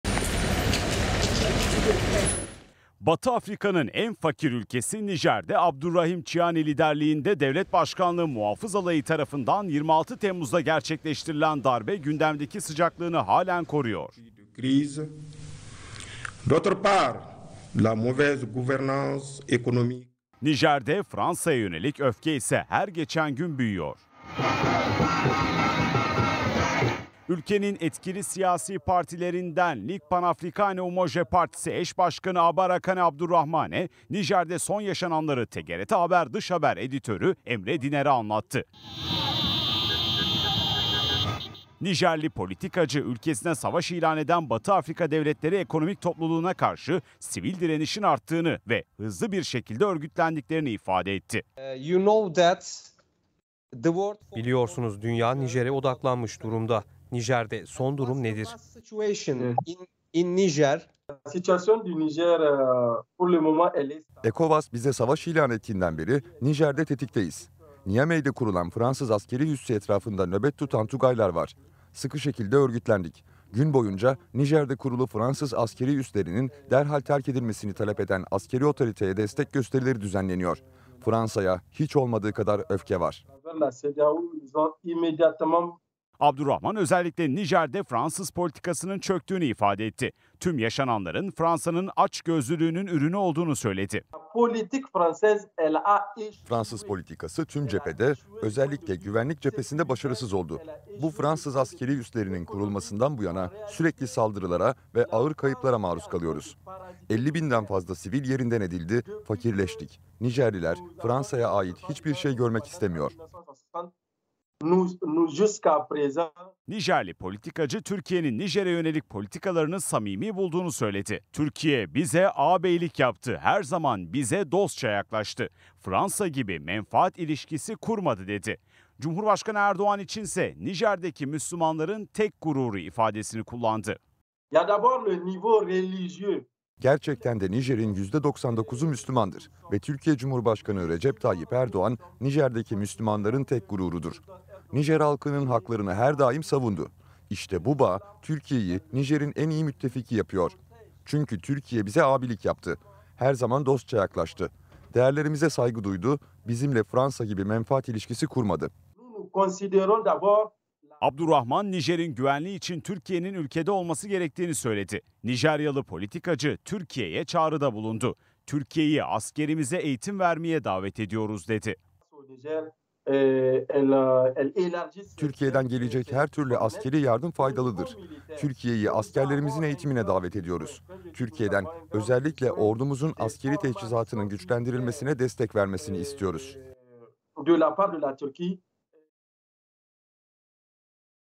Fransa Karşıtı Nijerli Politikacı TGRT Haber'e Konuştu: Fransa'ya Öfke Türkiye'ye Sevgi Var
Gözler, 26 Temmuz’da meydana gelen askeri darbenin ardından, eski Fransız sömürgesi olan Nijer’de yaşananlar da ülkede Fransız sömürüsüne karşıt "Ligu Panafrikan Umoja" partisi eş başkanı Kane Abdurahman, ülkedeki son durumu TGRT Haber'den Emre Diner’e anlattı. Abdurahman, "sivil halk Fransa’ya karşı örgütlendi ve Nijerlililer ülkede Fransa’ya ait hiç bir şey görmek istemiyor” dedi. Türkiye’yle ilgili ise, "bize ağabeylik yaptı" dedi. Nijerli politikacının Türkiye’ye bir de kritik çağrısı oldu.